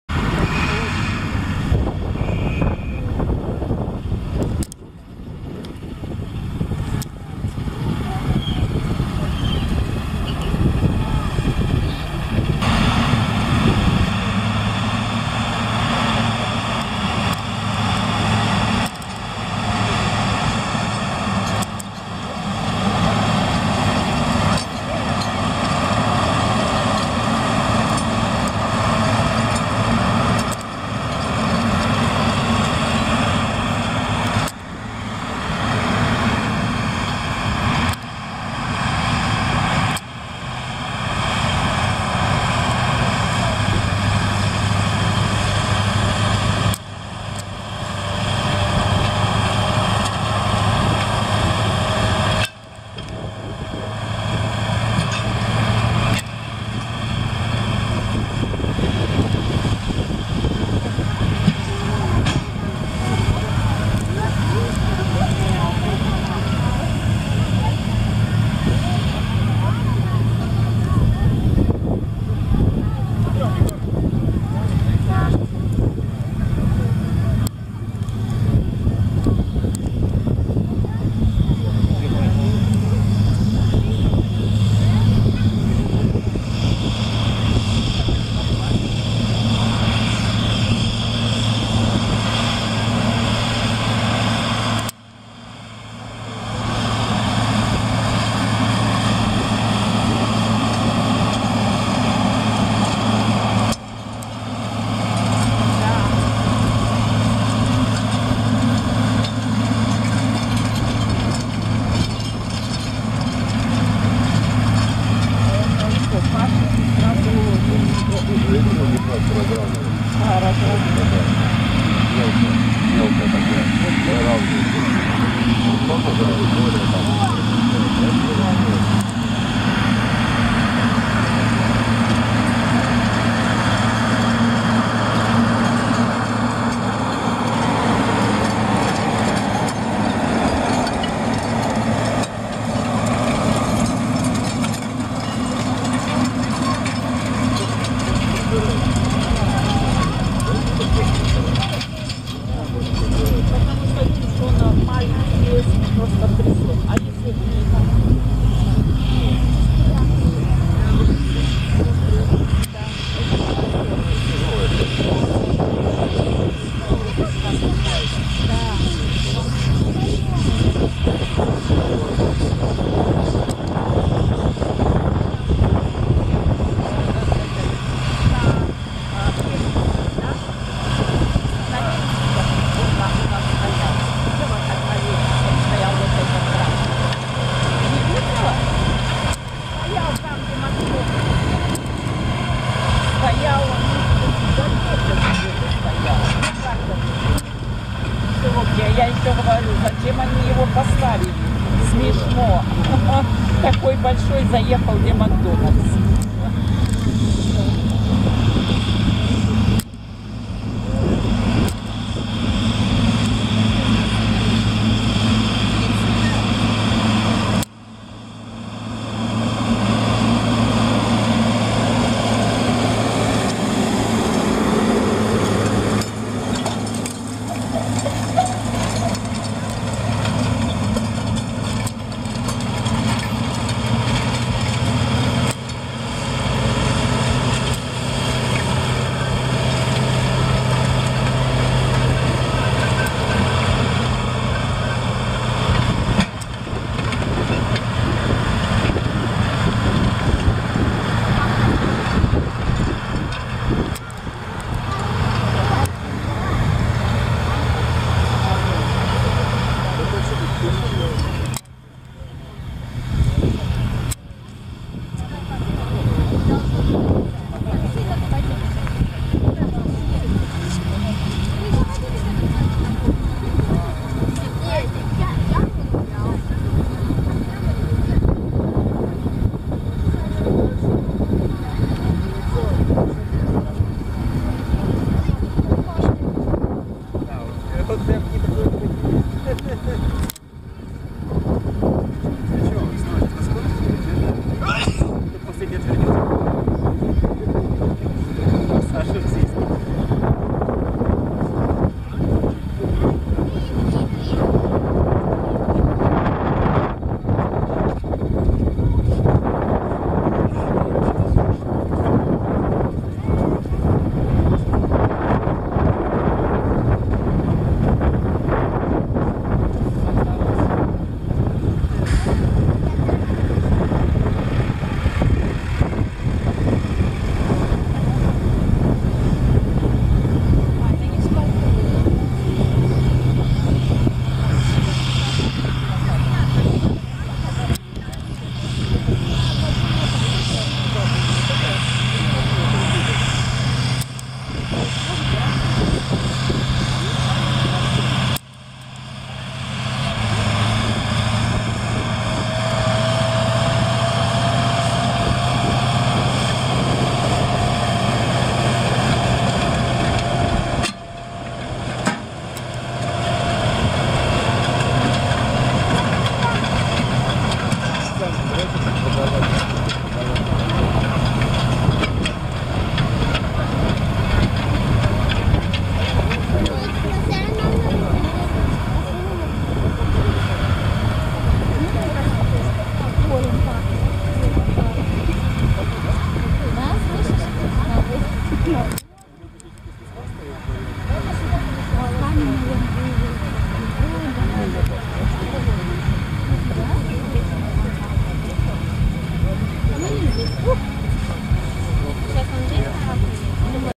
Кировец К 742МСт1 с дискатором и Кировец К 525Пр с лущильщиком
Демонстрационный показ работы тракторов Кировец.